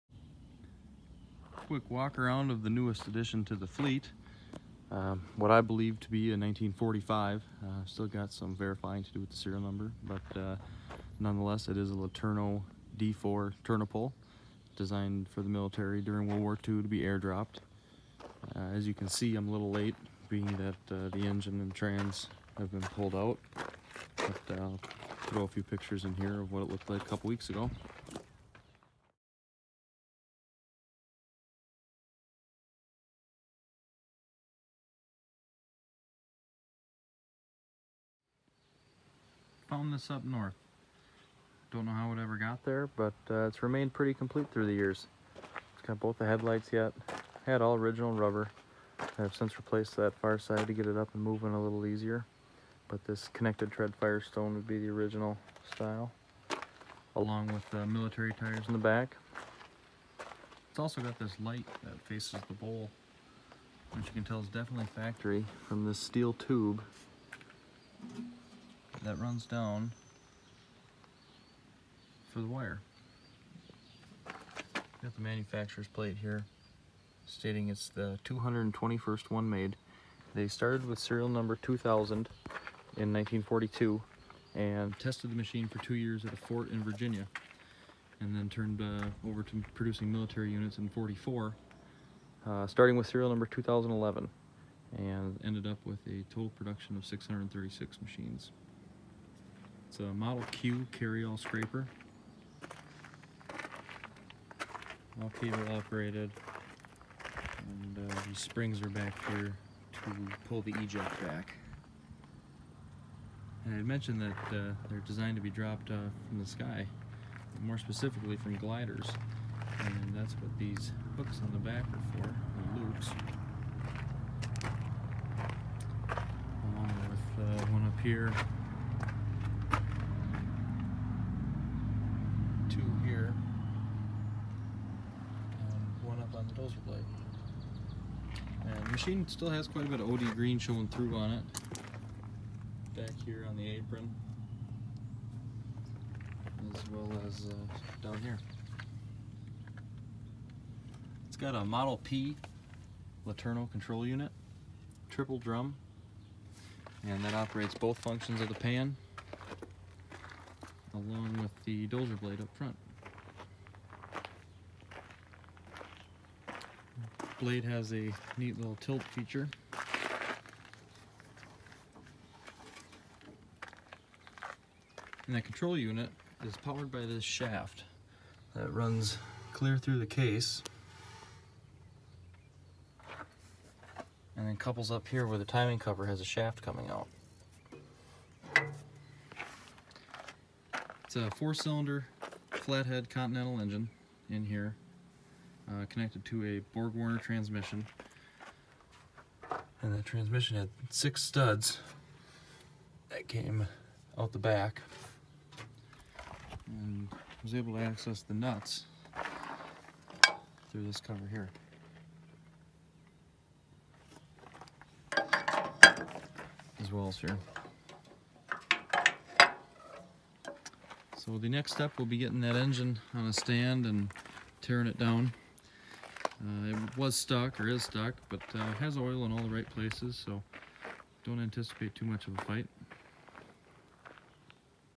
LeTourneau D4 airborne scraper
Quick overview of a new project. Engineered to be parachute dropped during WW2, the D4 tournapull was the smallest ever made by LeTourneau.